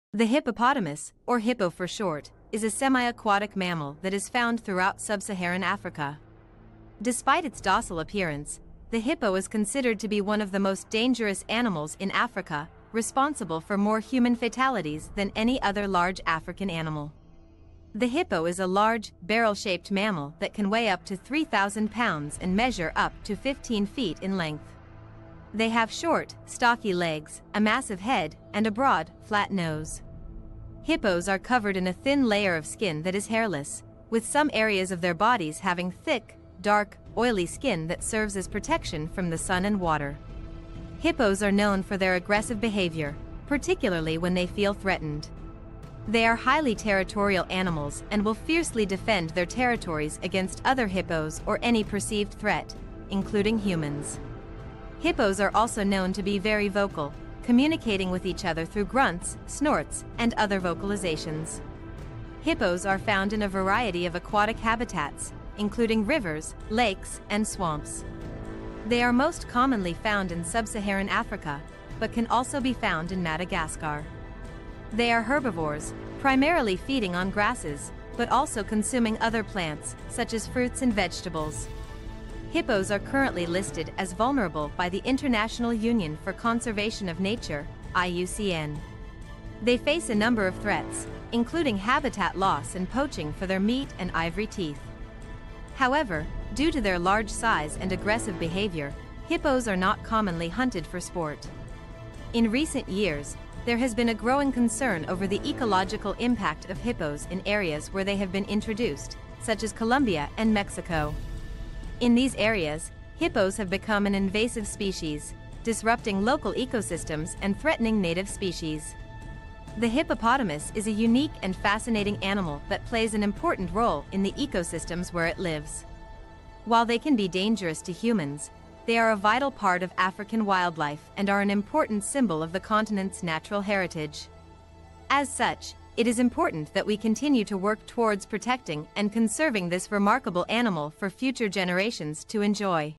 Hippopotamus - The Most Dangerous Animal On The River
The hippopotamus, or hippo for short, is a semi-aquatic mammal that is found throughout sub-Saharan Africa.